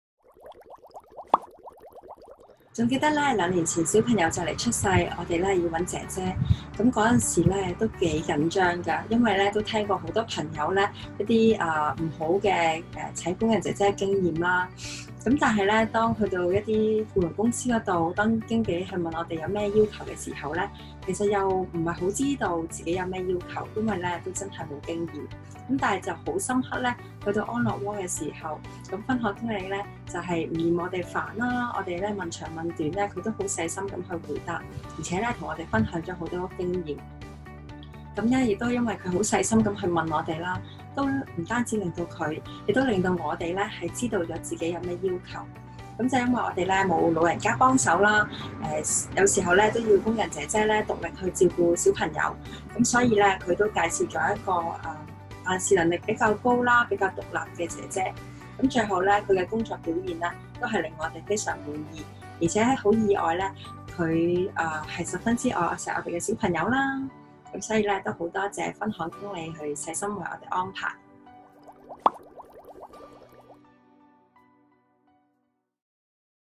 窩心客戶回饋
非常感激客戶 曹太一家對我們的信任和支持🥰
安樂窩會繼續承諾提供優質的配對服務💪
並挑選合適的外傭，為雙方建立長遠的僱傭關係💜